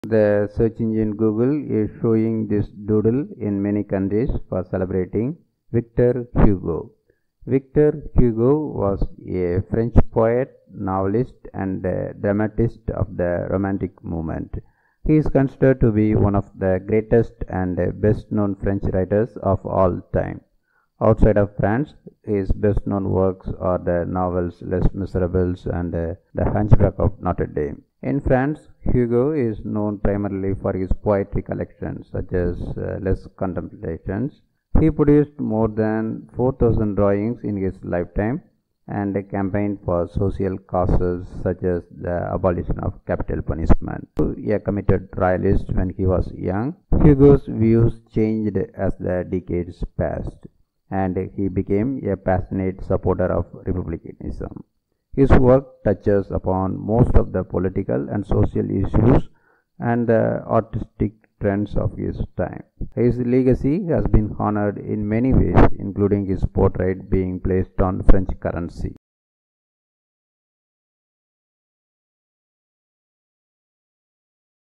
Victor Hugo Google Doodle. Short Biography of Víctor Hugo | QPT
The Search Engine Google is showing this Doodle in many countries for celebrating Victor Hugo.

Victor  Hugo  was a French poet, novelist, and dramatist of the Romantic movement. He is considered to be one of the greatest and best-known French writers of all time. Outside of France, his best-known works are the novels Les Misérables, , and The Hunchback of Notre-Dame . In France, Hugo is known primarily for his poetry collections, such as Les Contemplations . He produced more than 4,000 drawings in his lifetime, and campaigned for social causes such as the abolition of capital punishment.

Though a committed royalist when he was young, Hugo's views changed as the decades passed, and he became a passionate supporter of republicanism;  his work touches upon most of the political and social issues and the artistic trends of his time.  His legacy has been honoured in many ways, including his portrait being placed on French currency.